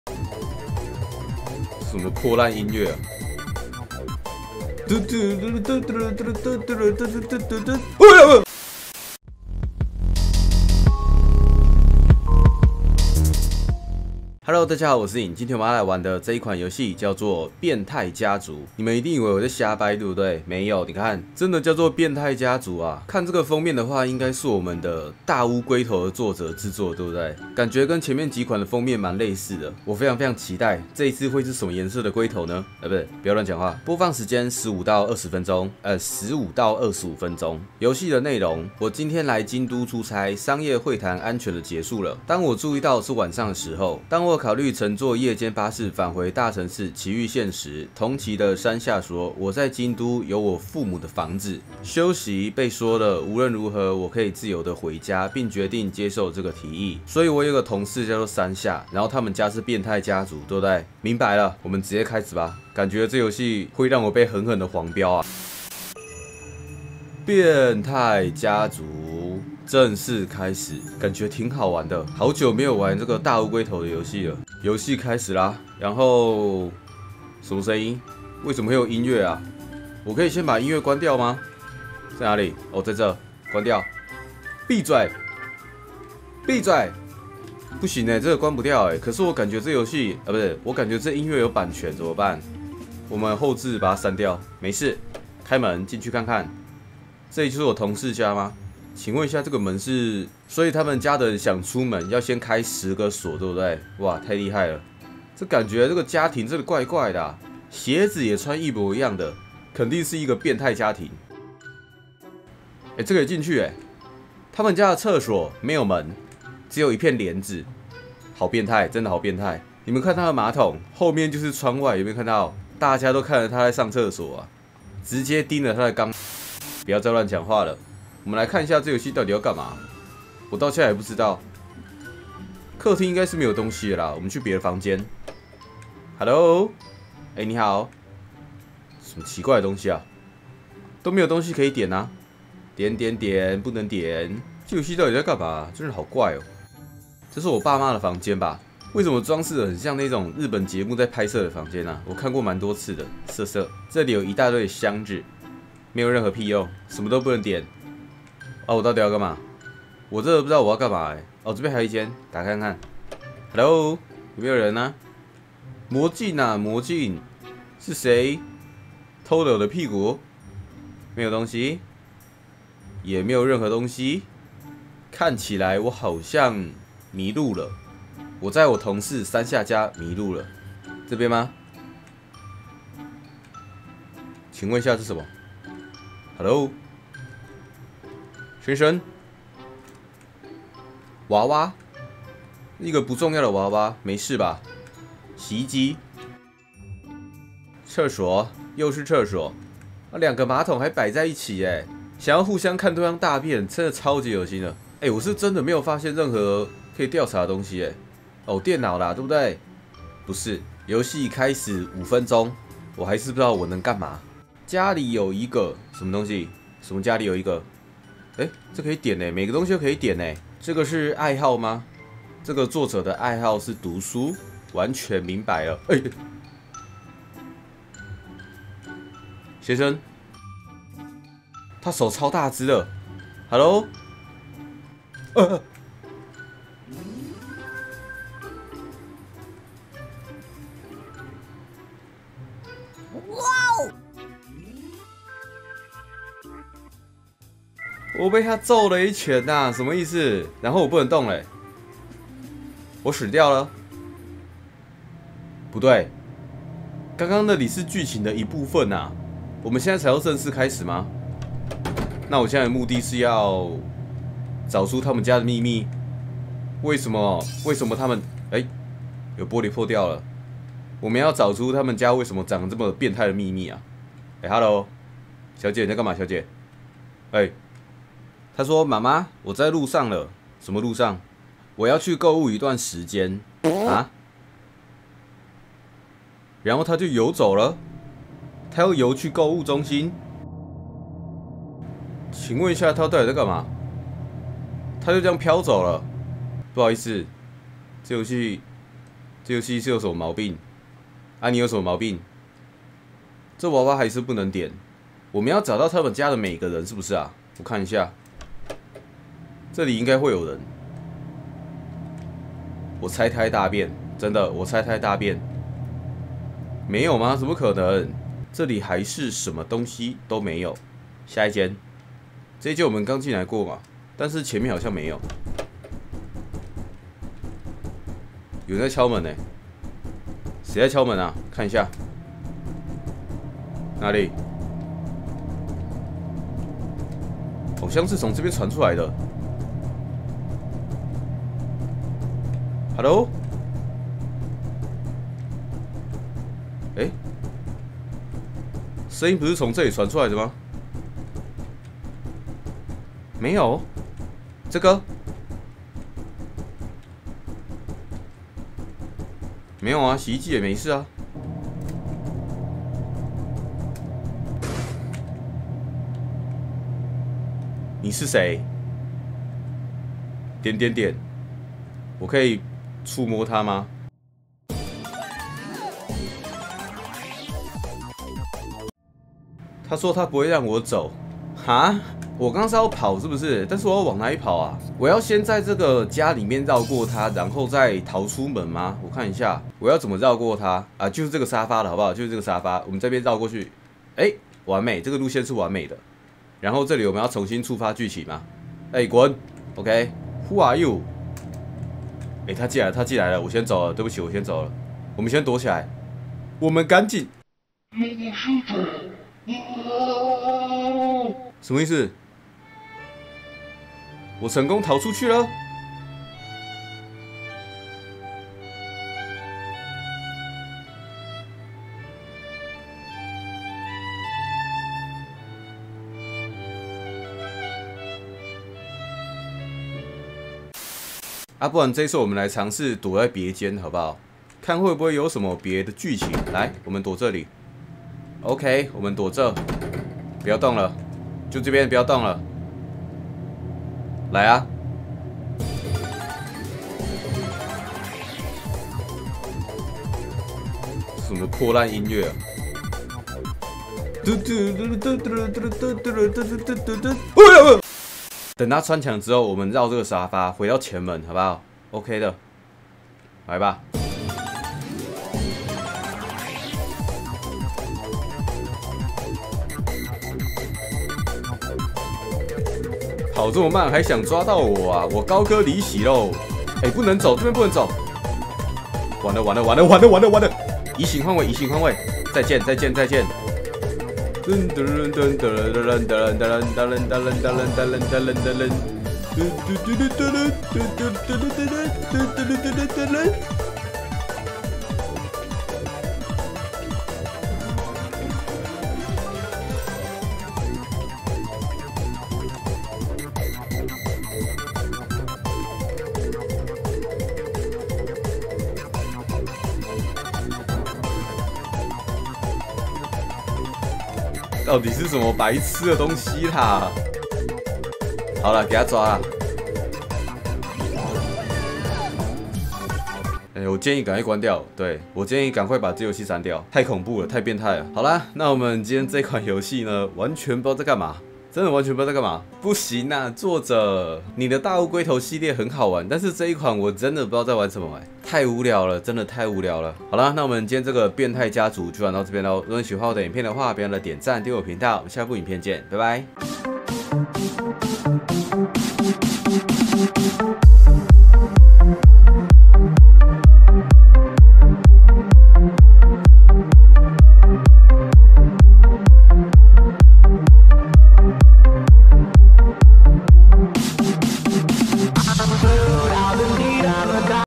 這是一款真正的色情gay遊戲...同事:我一個人住，我的房子還蠻大的，歡迎你們來我家玩，玩累了就直接睡覺，沒問題的。【恐怖爆笑遊戲】
87分。
有任何建議或想說的話都歡迎在下方留言告訴我 ~
未來會持續上傳更多不同類型的遊戲影片
也可直接使用"超級感謝"留言~